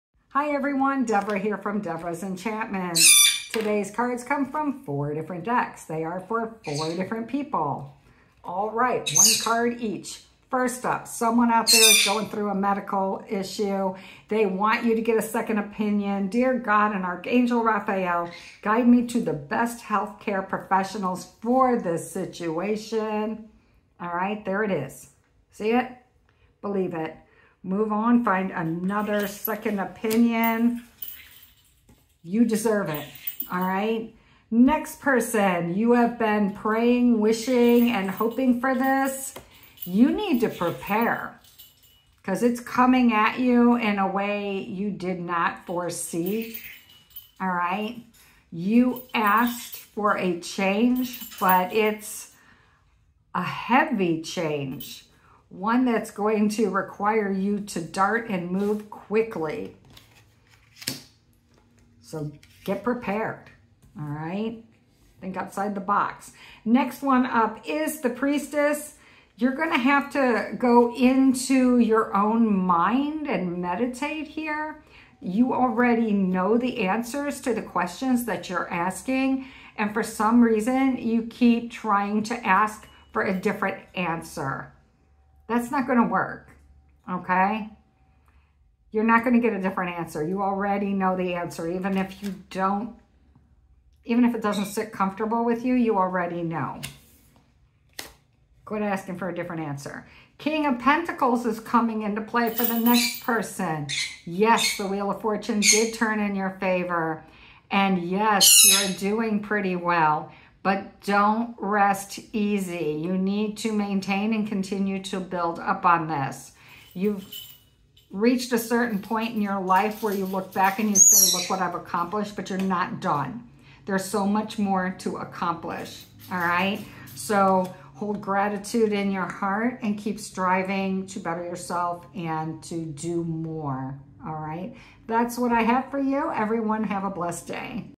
4 cards 4 4 people! See what I did there? 444 😊
Today's cards come from four different decks for four different people. Enjoy!